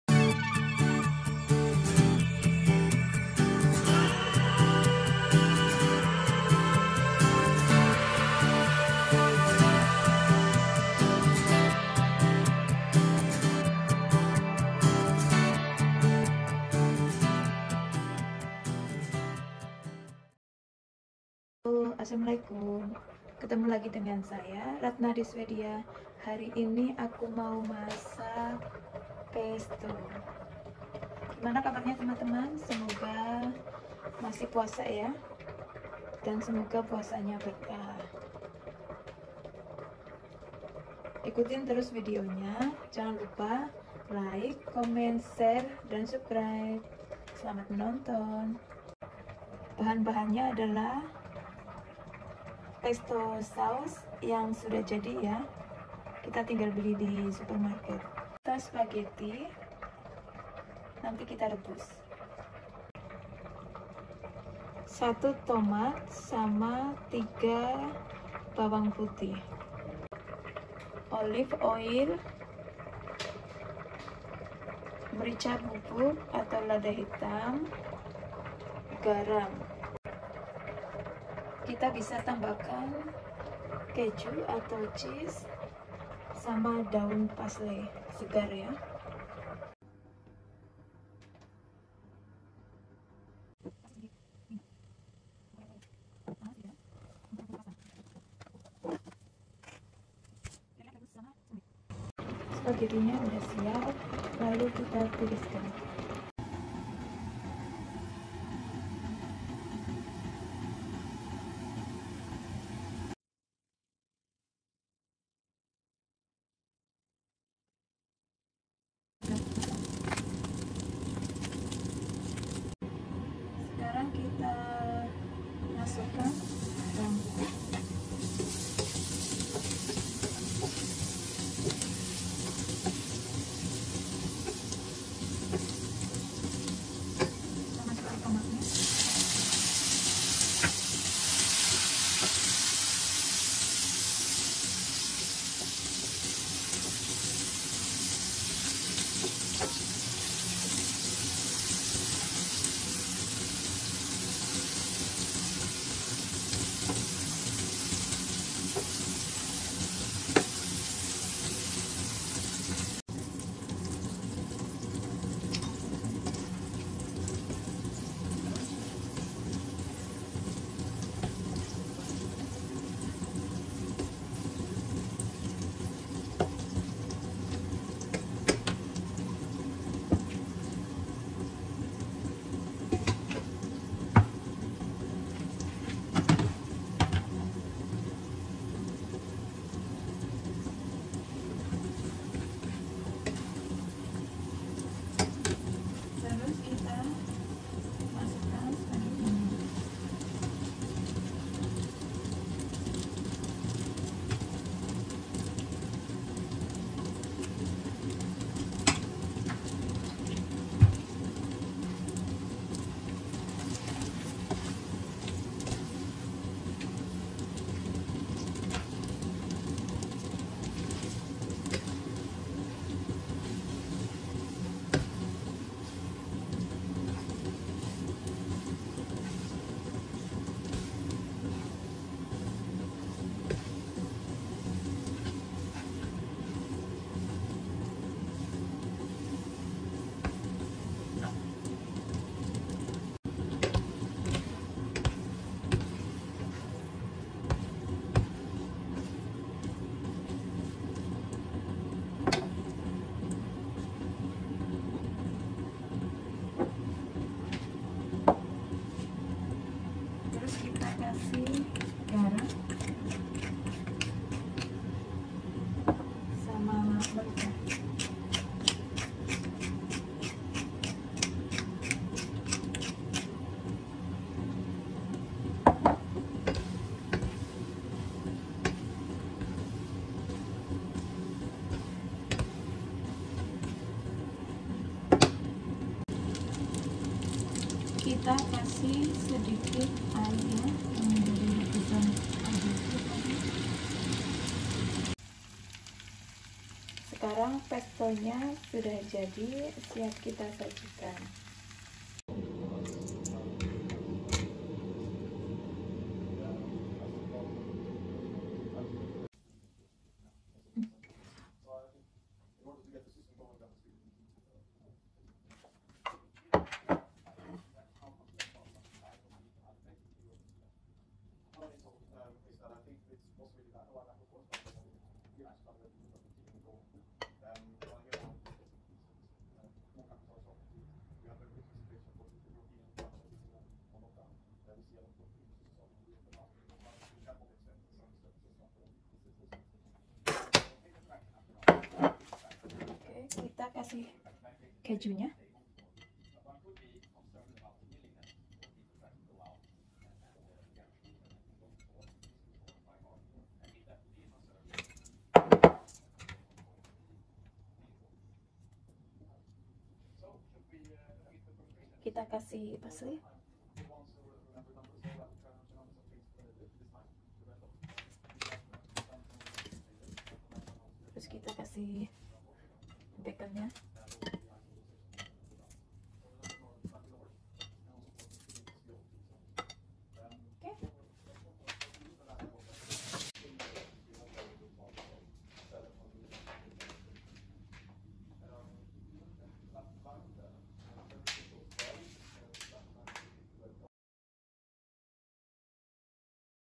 Pesto Pasta, super praktis, cepat dan enak. 20200514
Pesto Pasta Recepi
2 1/2 tbsp pesto
1 cloves garlic
1 tomato
2 tbsp olive oil
2 tbsp parmesan cheese
Pasta

Directions:
1. Cook pasta in a large pot of boiling water until done. Drain.
2. Meanwhile, heat the oil in a frying pan over medium low heat. Add garlic, tomatoes, pesto, salt and pepper.
Cook about 5 minutes, or until tomatoes are soft.
3 Add pasta mix well into mix pesto mixture.
Serve with cheese, fresh parsley and bacon (can be replace)